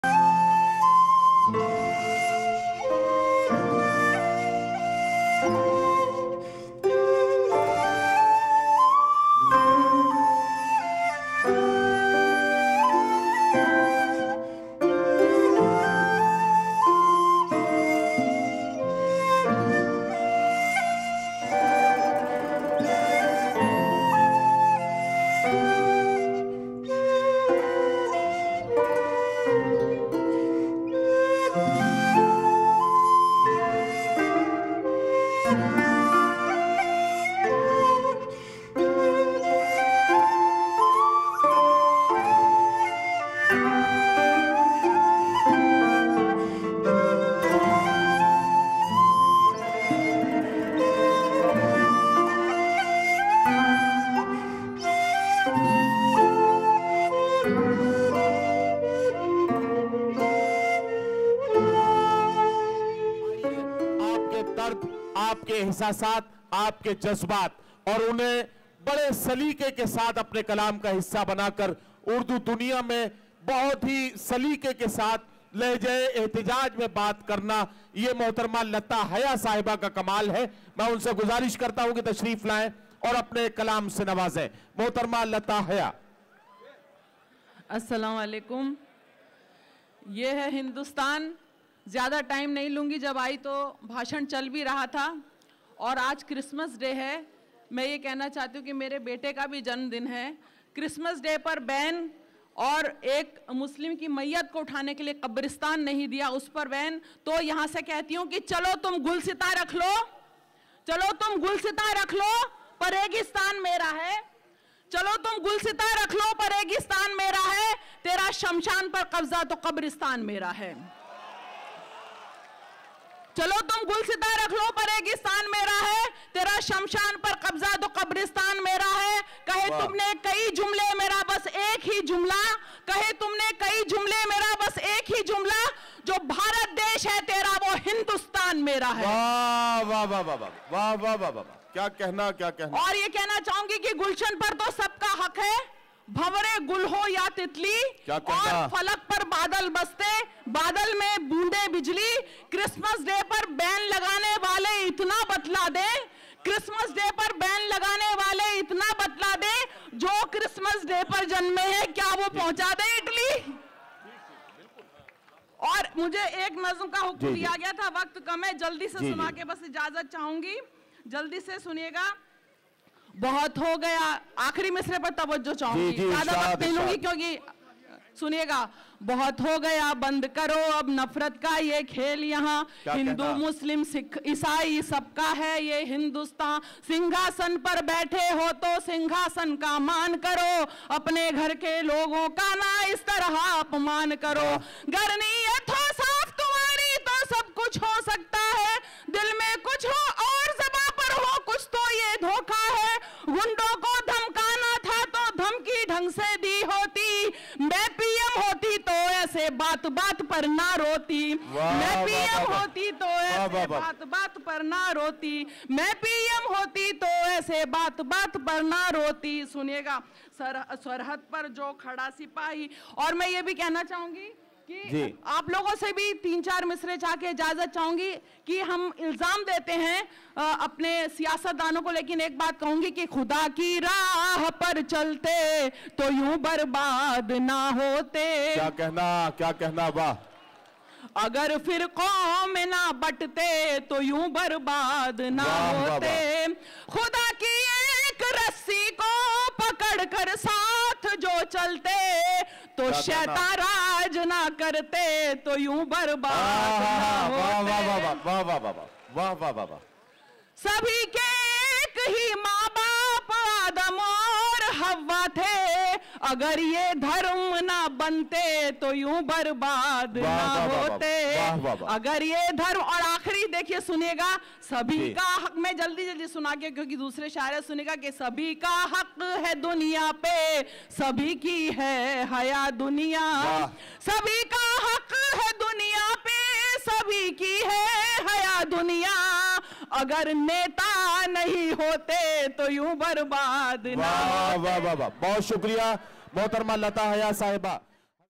Lata Haya ||जो भारत देश है तेरा तो हिन्दुस्तान मेरा है ||Mumbra Mushaira 25/12/2017
AENA MUSHAIRA... Paasban-E-Adab Presents KUL HIND MUSHAIRA on Sunday 25th December 2017, at Mittal ground, Mumbra Thane. Organsers are Shamim Ahmed khan, Ashraf U Shanu Pathan,Dr. M Shah, Bablu Sayyed, Imtiyaz Khan ,Sonu Hashmi, Sufiyan Guddu And Imtiyaz Vanoo.